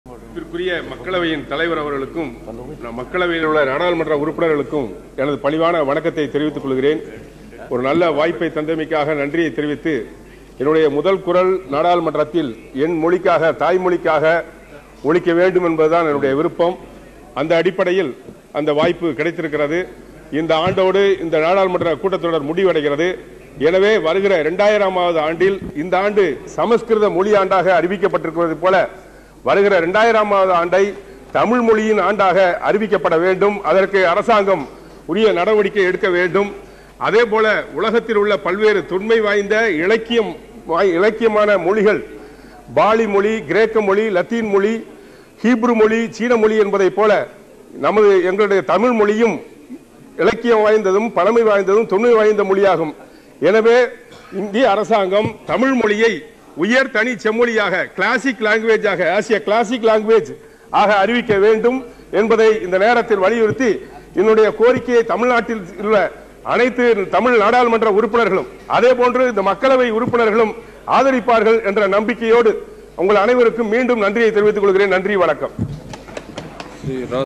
தமிழைச் செம்மொழியாக அறிவிக்கக் கோரிய தோழர் பி.மோகன் நினைவு தினம்
தமிழைச் செம்மொழியாக அறிவிக்கக் கோரி 1999 ஆம் ஆண்டில் தோழர் பி.மோகன் நாடாளுமன்றத்தில் தமிழிலேயே ஆற்றிய உரை. இன்று தோழர் பி.மோகன் அவர்களின் நினைவு தினம் (30 December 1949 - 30 October 2009).